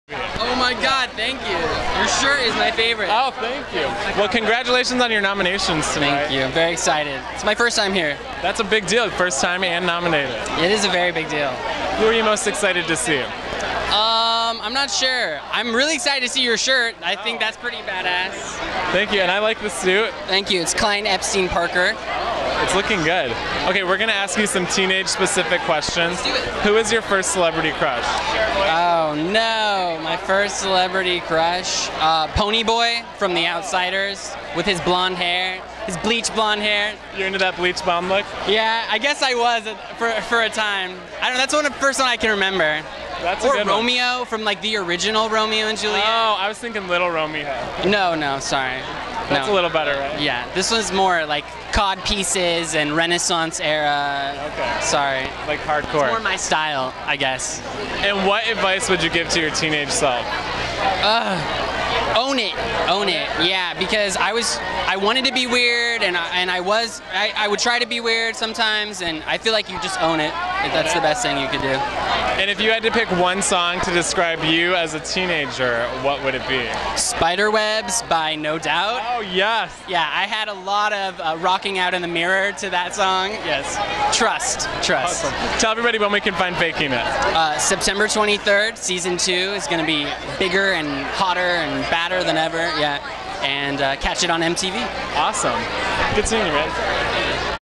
Michael Willett @ The Teen Choice Awards | AfterBuzz TV
AfterBuzz TV Host, Jason Ikeler interviews Michael Willett at the 2014 Teen Choice Awards.